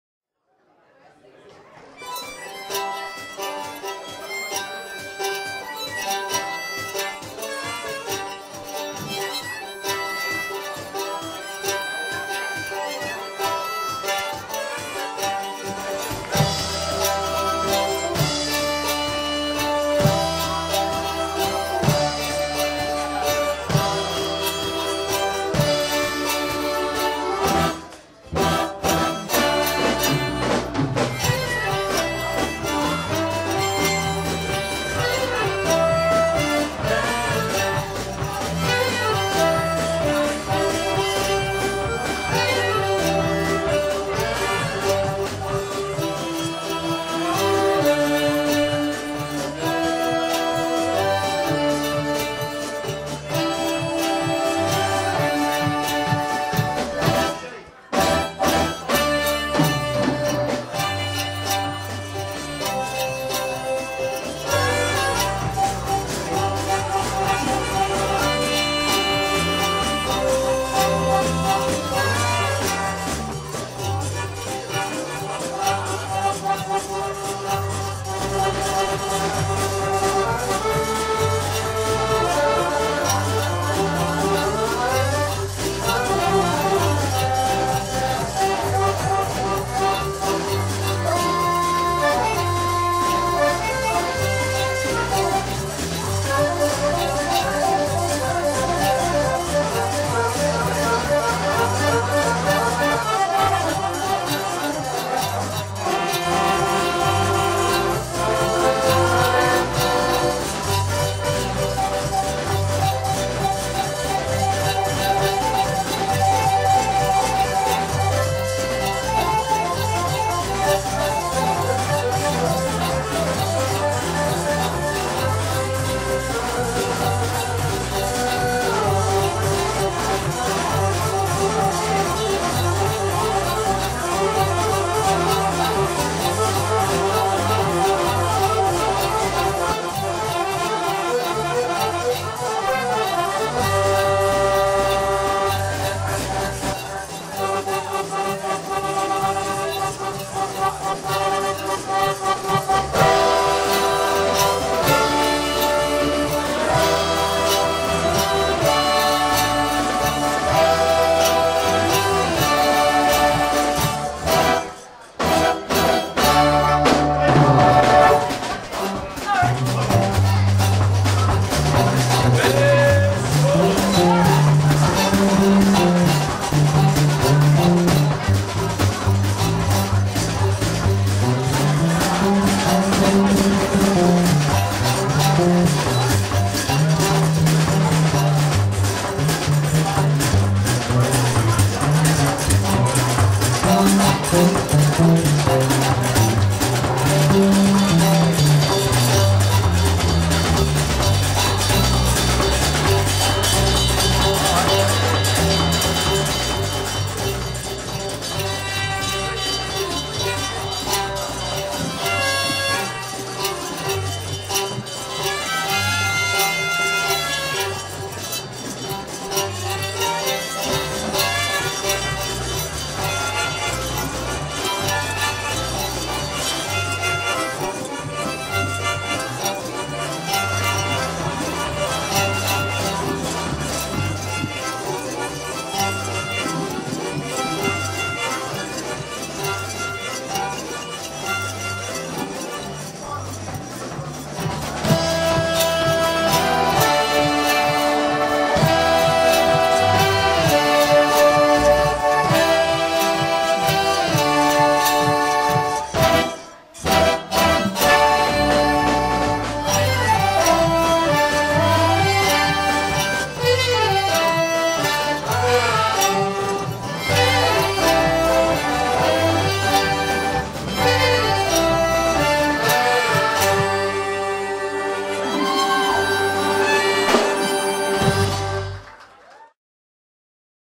Zydeco Jump 2
Part of Tooth Faerie DVD. Zydeco Jump play Louisiana dance music in Melbourne.